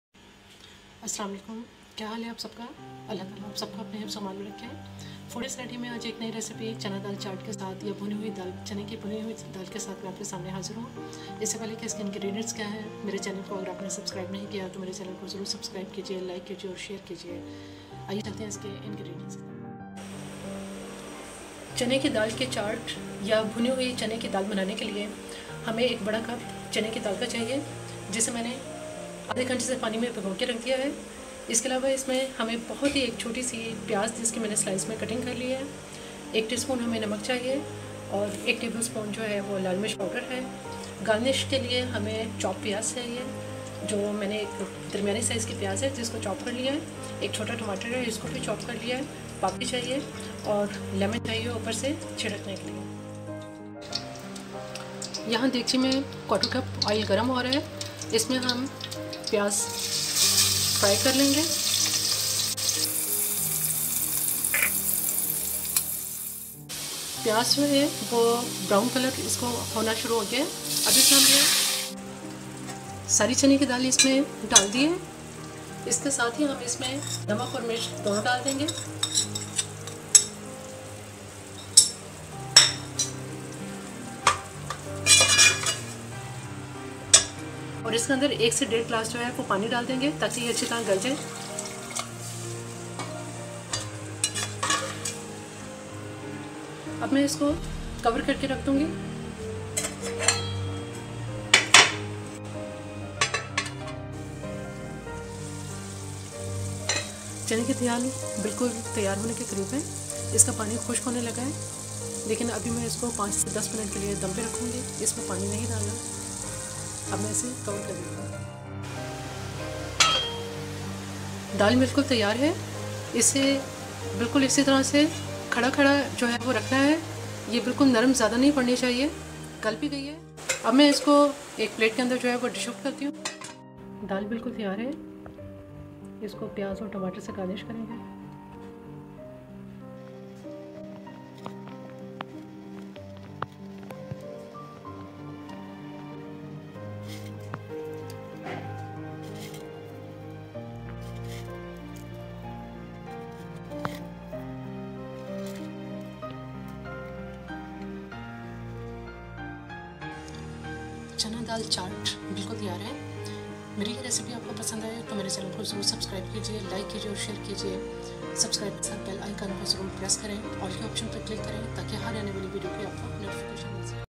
Chana daal chaat | chat recipe for Ramadan | Food iz ready
Assalam-o-alakum

Chana daal chaat is very different and tasty chaat. Its taste is very unique and amazing. Its a very tasty snack for Ramadan. Its ingredients are Chana daal onion, tomato, lemon, salt & pepper.